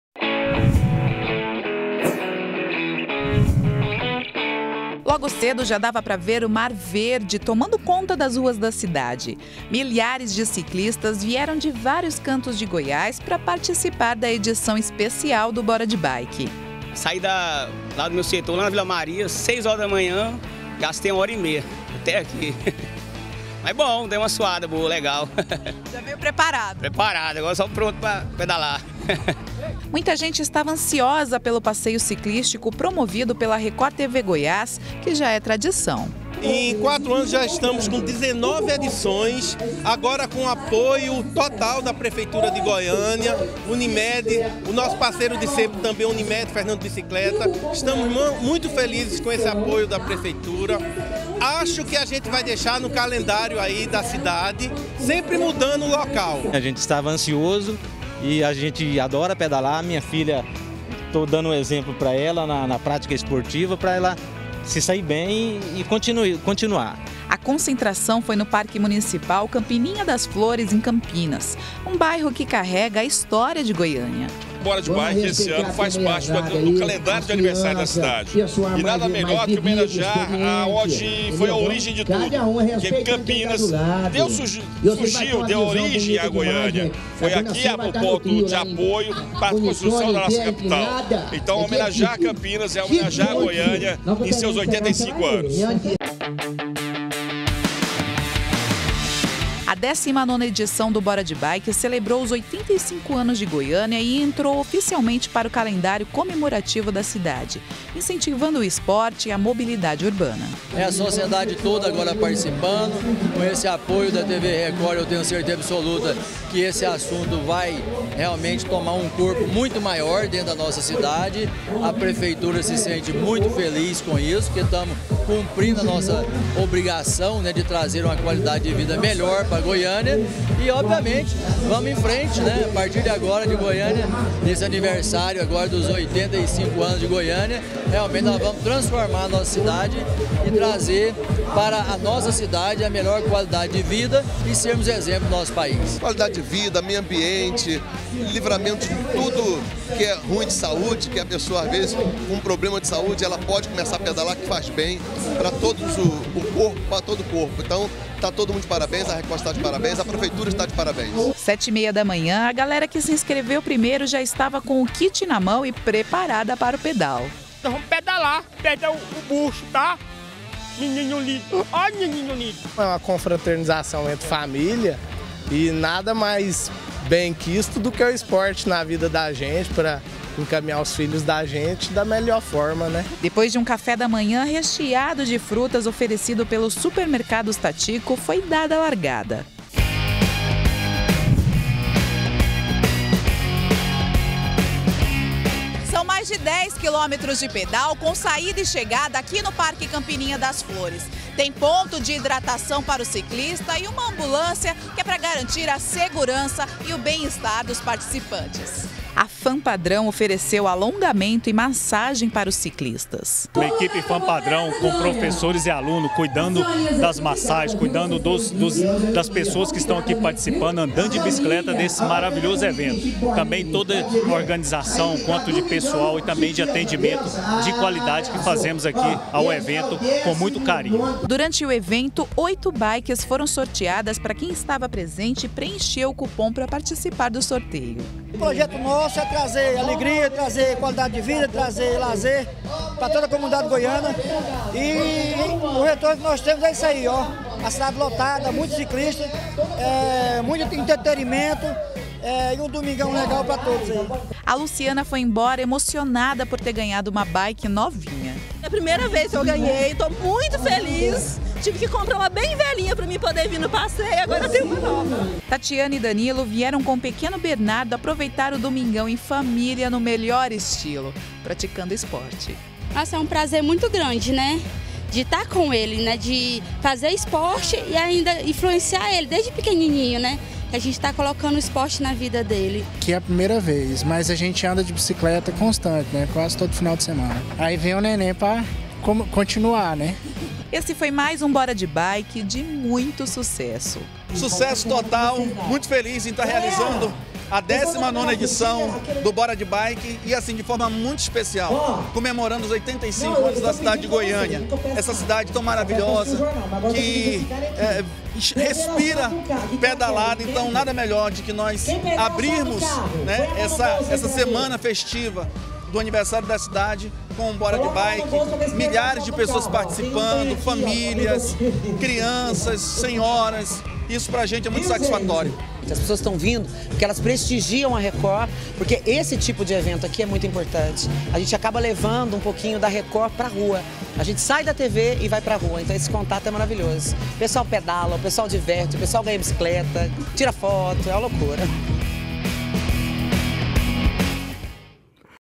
BG - 19º BORA DE BIKE comemora os 85 anos de Goiânia - 22-10-2018
A 19ª edição do passeio ciclístico BORA DE BIKE foi um sucesso. E dessa vez o evento promovido pela Record TV Goiás, em parceria com a prefeitura de Goiânia, Fernando Bicicletas e Unimed Goiânia, fez parte das comemorações do aniversário de 85 anos da nossa capital.